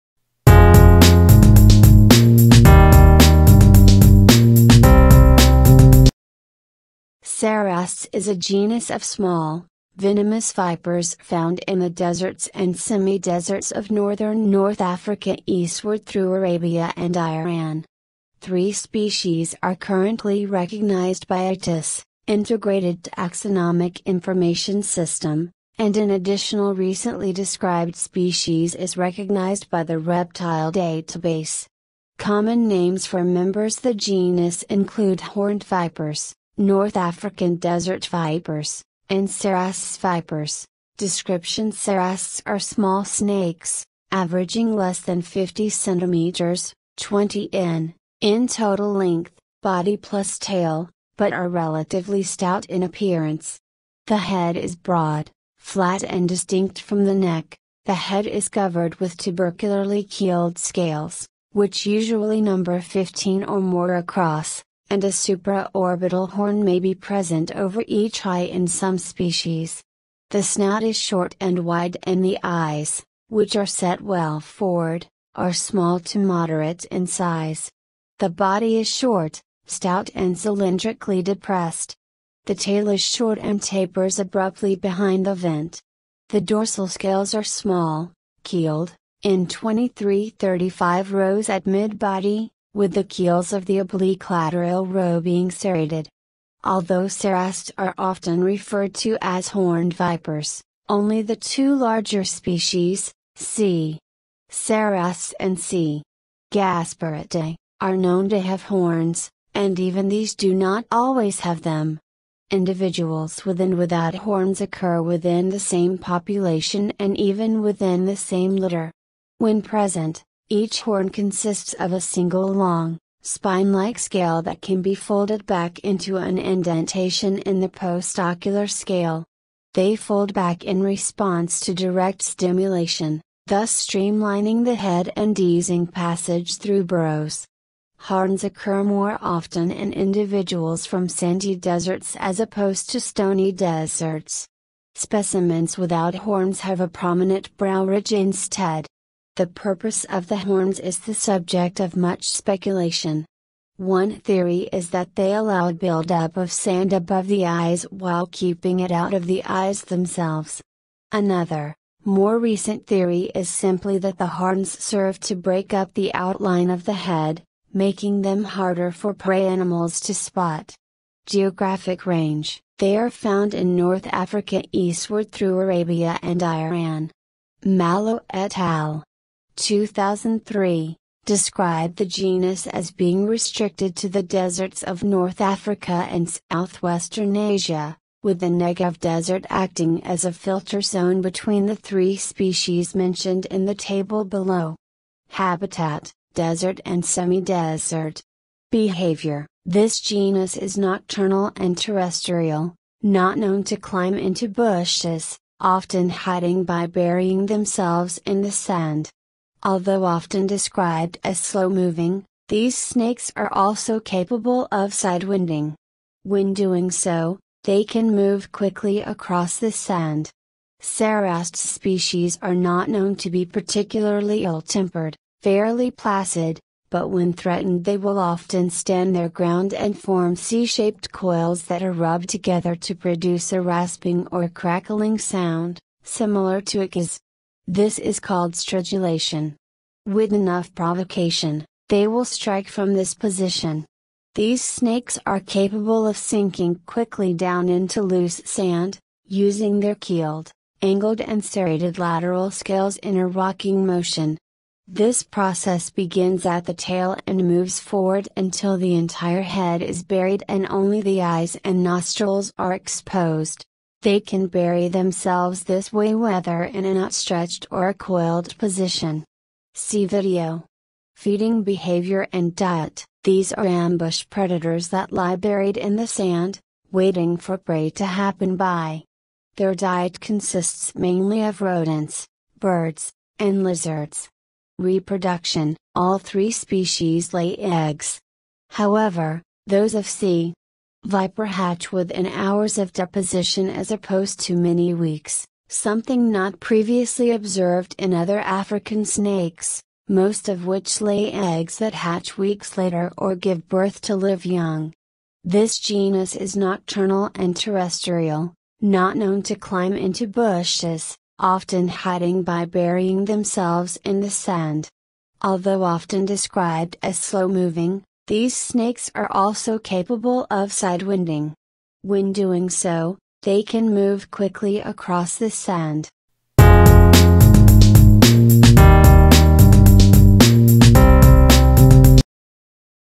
Cerastes genus
Cerastes is a genus of small, venomous vipers found in the deserts and semi-deserts of northern North Africa eastward through Arabia and Iran. Three species are currently recognized by "ITIS" (Integrated Taxonomic Information System), and an additional recently described species is recognized by the Reptile Database. Common names for members the genus include horned vipers, North African desert vipers, and cerastes vipers.

Description
Cerastes are small snakes, averaging less than 50 cm (20 in) in total length (body + tail), but are relatively stout in appearance. The head is broad, flat and distinct from the neck. The head is covered with tubercularly keeled scales, which usually number 15 or more across, and a supraorbital horn may be present over each eye in some species. The snout is short and wide and the eyes, which are set well forward, are small to moderate in size. The body is short, stout and cylindrically depressed. The tail is short and tapers abruptly behind the vent. The dorsal scales are small, keeled, in 23-35 rows at midbody, with the keels of the oblique lateral row being serrated.

Although Cerastes are often referred to as horned vipers, only the two larger species, C. cerastes and C. gasperettii, are known to have horns, and even these do not always have them. Individuals with and without horns occur within the same population and even within the same litter.

When present, each horn consists of a single long, spine-like scale that can be folded back into an indentation in the postocular scale. They fold back in response to direct stimulation, thus streamlining the head and easing passage through burrows. Horns occur more often in individuals from sandy deserts as opposed to stony deserts. Specimens without horns have a prominent brow ridge instead.

The purpose of the horns is the subject of much speculation. One theory is that they allow a buildup of sand above the eyes while keeping it out of the eyes themselves. Another, more recent theory is simply that the horns serve to break up the outline of the head, making them harder for prey animals to spot.
Geographic range

They are found in North Africa eastward through Arabia and Iran. Mallow et al. (2003) describe the genus as being restricted to the deserts of North Africa and southwestern Asia, with the Negev desert acting as a filter zone between the three species mentioned in the table below.
Habitat

Desert and semi-desert.
Behavior

This genus is nocturnal and terrestrial (not known to climb into bushes), often hiding by burying themselves in the sand. Although often described as slow moving, these snakes are also capable of sidewinding. When doing so, they can move quickly across the sand.

Cerastes species are not known to be particularly ill-tempered ("fairly placid"), but when threatened they will often stand their ground and form C-shaped coils that are rubbed together to produce a rasping or crackling sound, similar to Echis. This is called stridulation. With enough provocation, they will strike from this position.

These snakes are capable of "sinking" quickly down into loose sand, using their keeled, angled and serrated lateral scales in a rocking motion. This process begins at the tail and moves forward until the entire head is buried and only the eyes and nostrils are exposed. They can bury themselves this way whether in an outstretched or a coiled position. See video.
Feeding behavior and diet